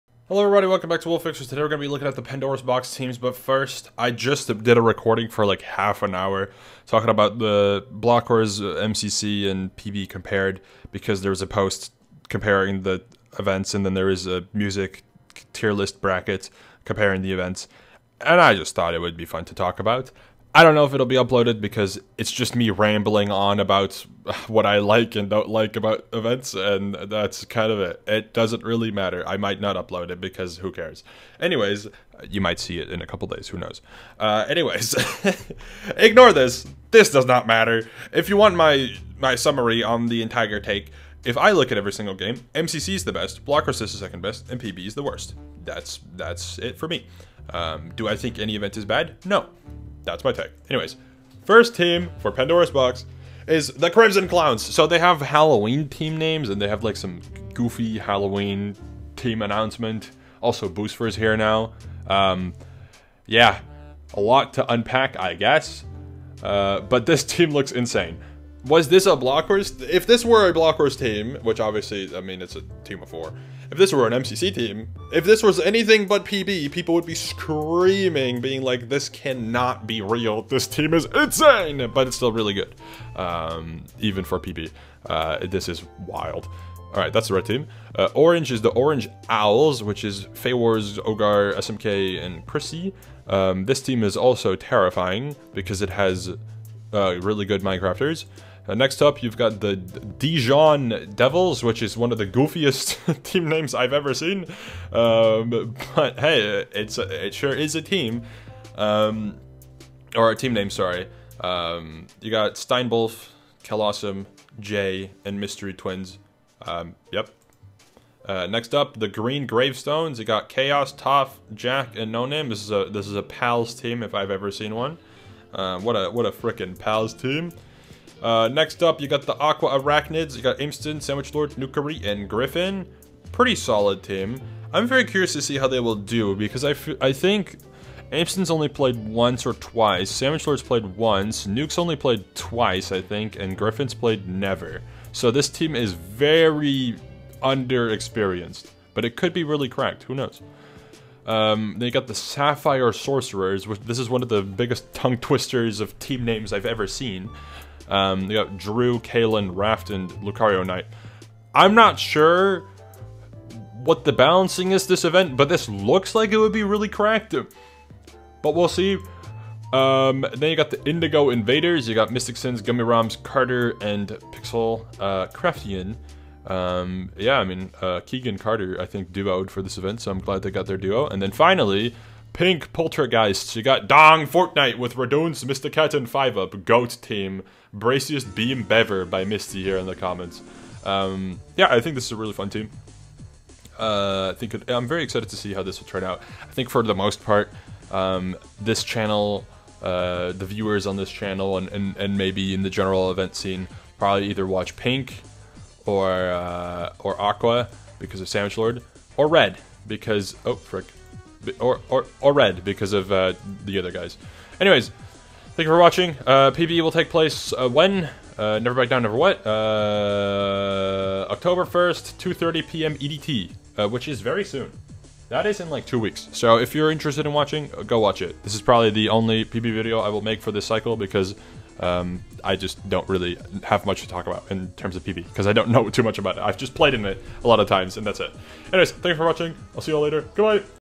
Pandora's Box 9 Teams ANNOUNCED!
The teams for Pandora's Box 8 have been announced! This event should features some familiar faces from MCC, Block Wars & Block Wars Origins! :D